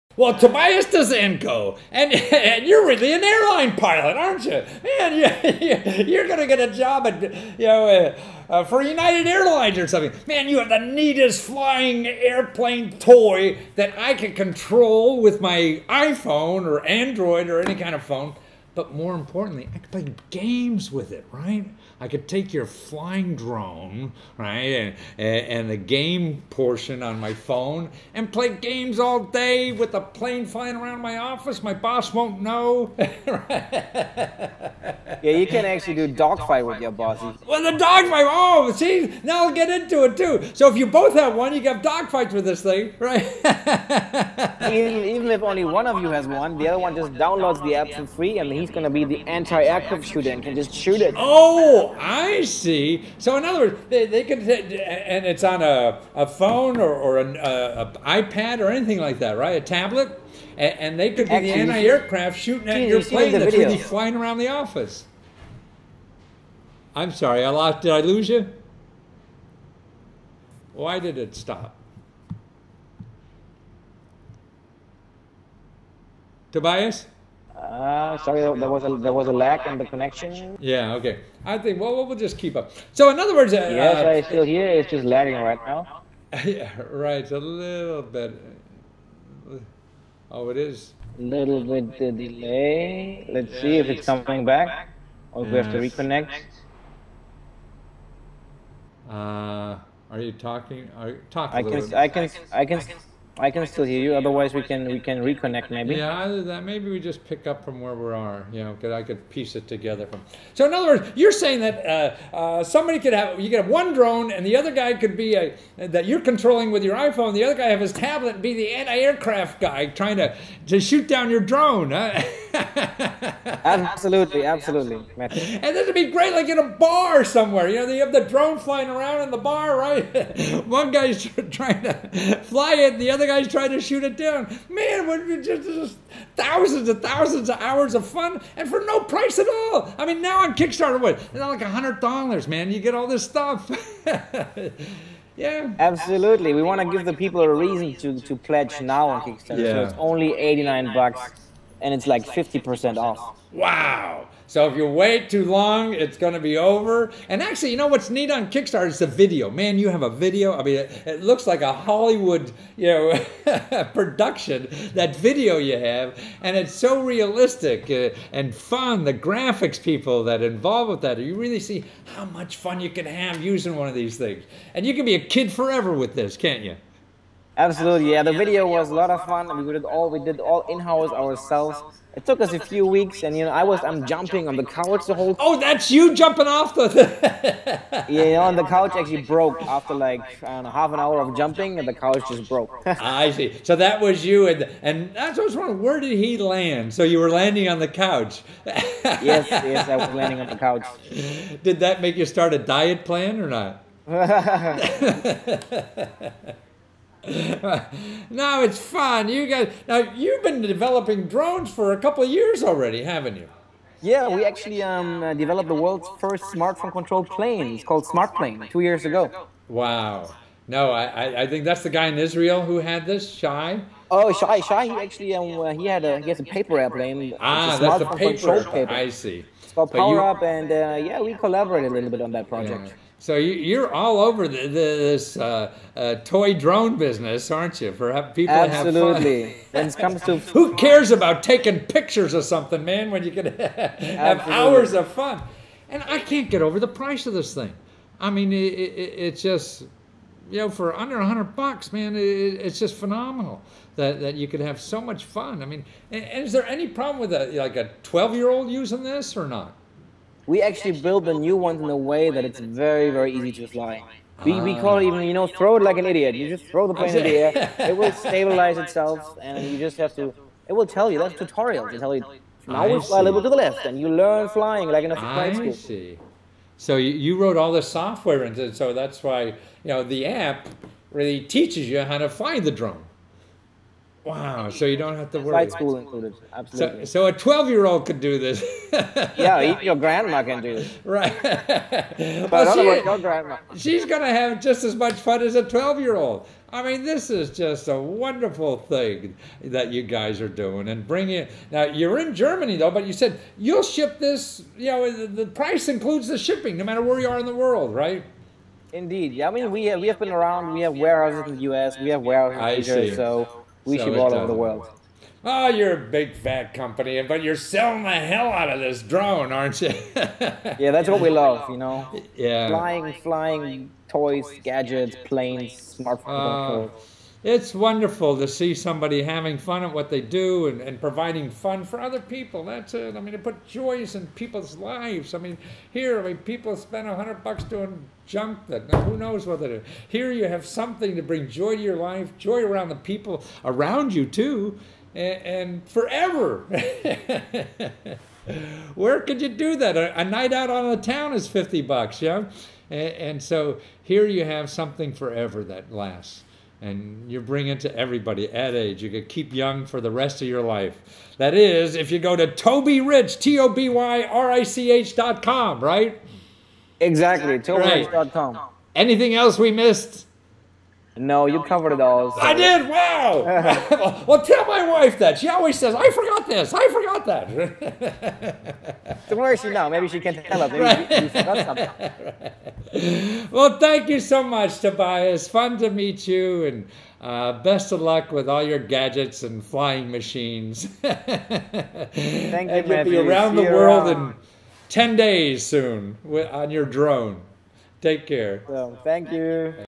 WOW: It's A Toy! It's A Game! It's Your Own Drone For Under $100
Matthew Lesko of interviews Tobias Danzenko of about the world´s first smartphone controlled gaming drone.